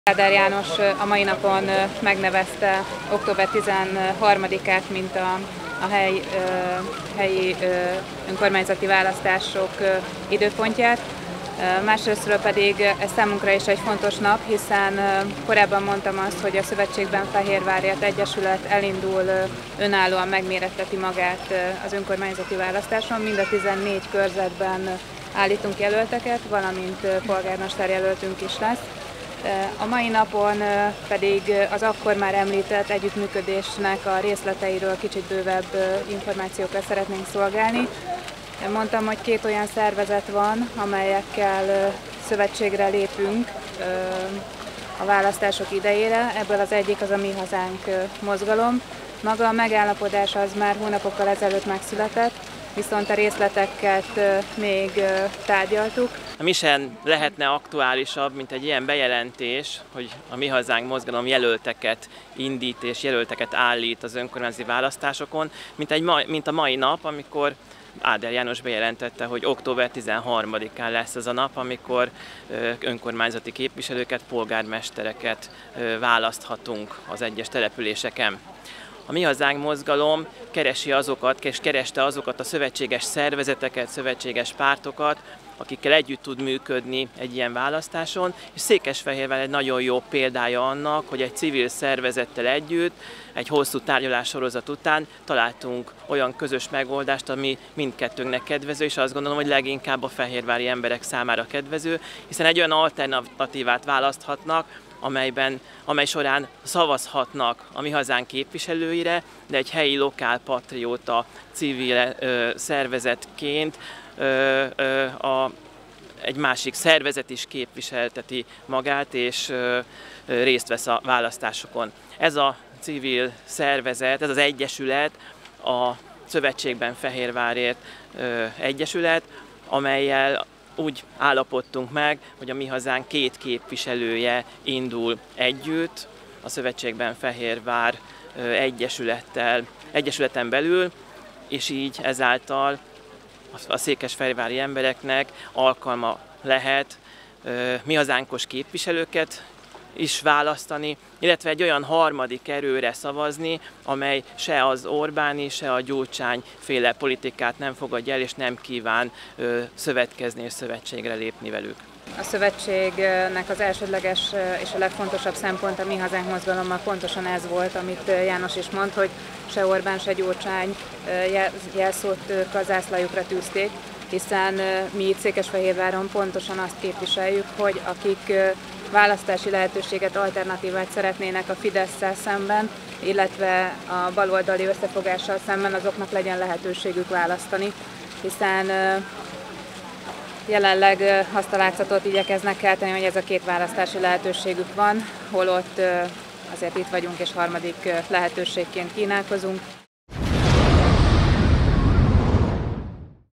Bemutattuk székesfehérvári polgármesterjelöltünket
Árgyelán János pártigazgató és Tancsa Ágnes polgármesterjelölt (Szövetségben Fehérvárért) közös sajtótájékoztatója (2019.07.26.)